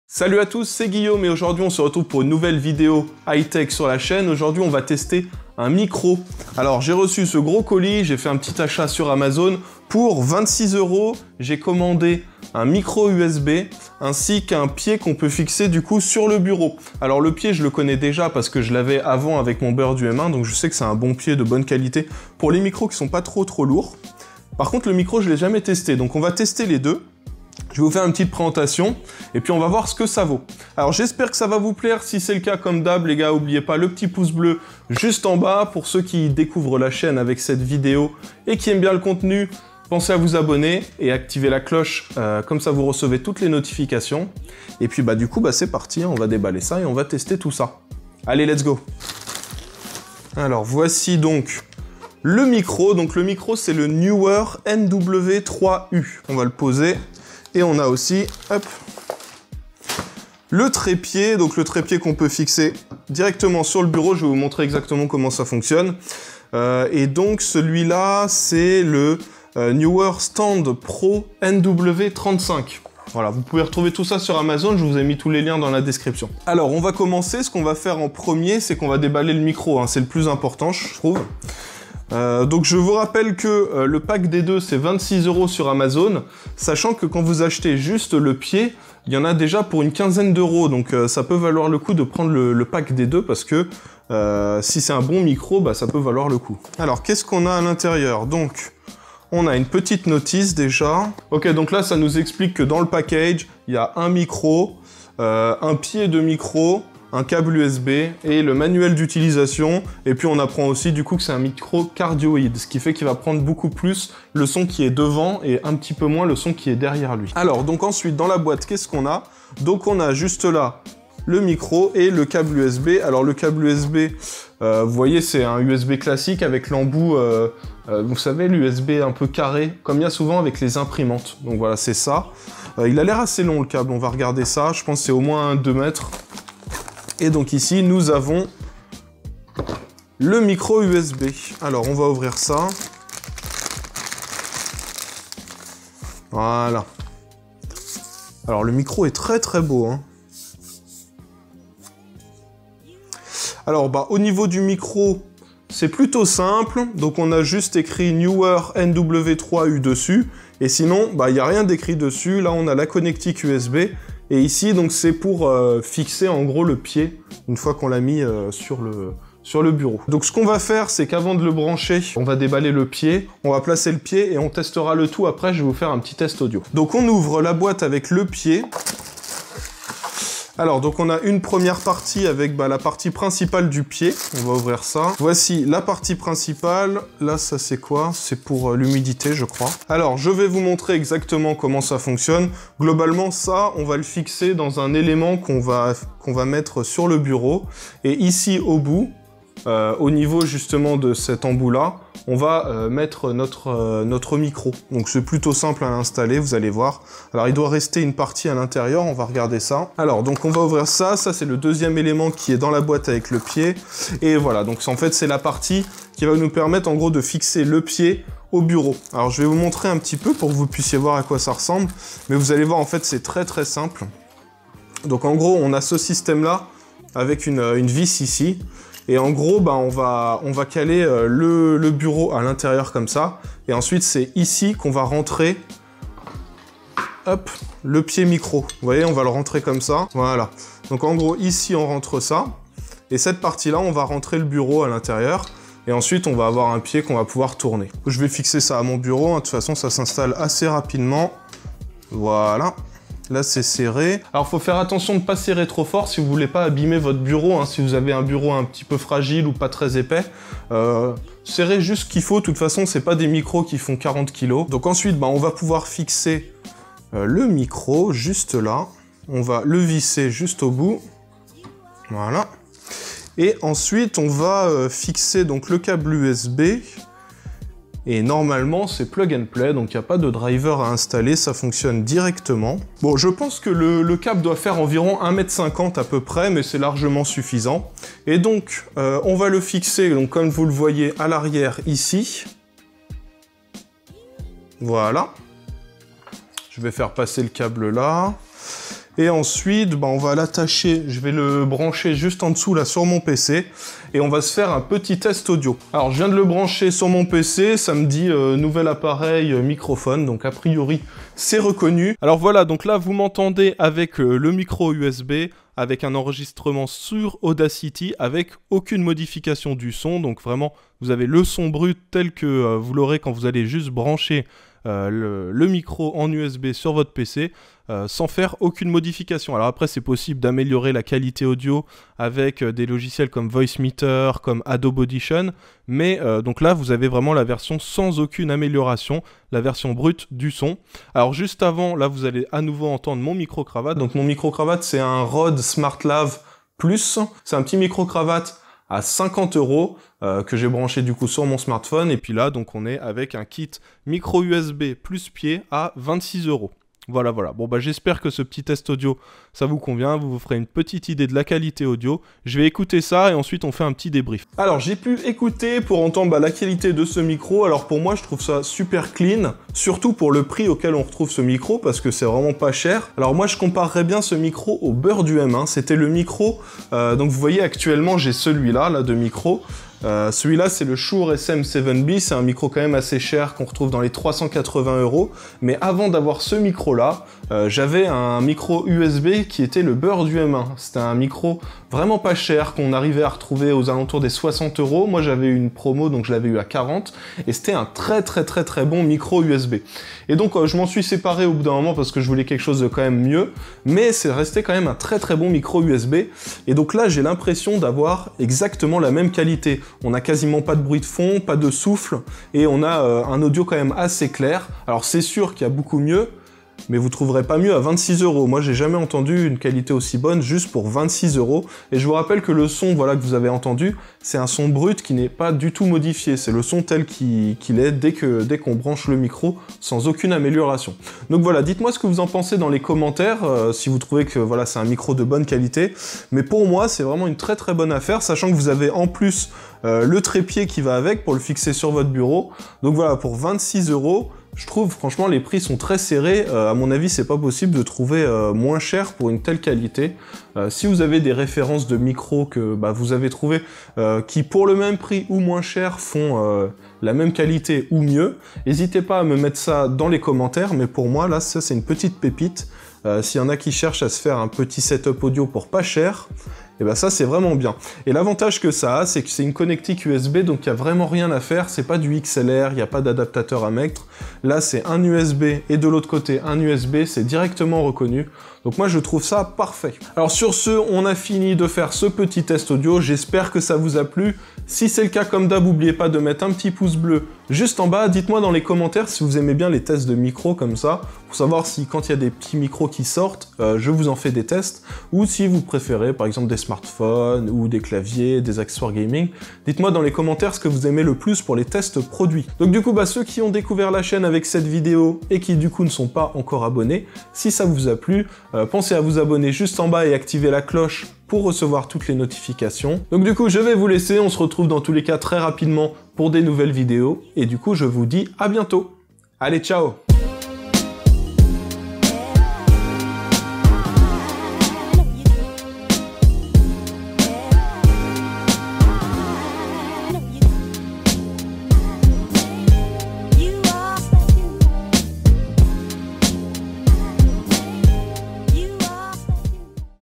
LE MEILLEUR MICRO USB PAS CHER - NEEWER NW-3U
Aujourd'hui, je vous présente un bundle Micro + Pied à 26€ que j'ai trouvé sur Amazon ! A ce prix là, je m'attendais pas du tout à une aussi bonne qualité !

Pack micro + pied

► S N A P C H A T : yt_guillaume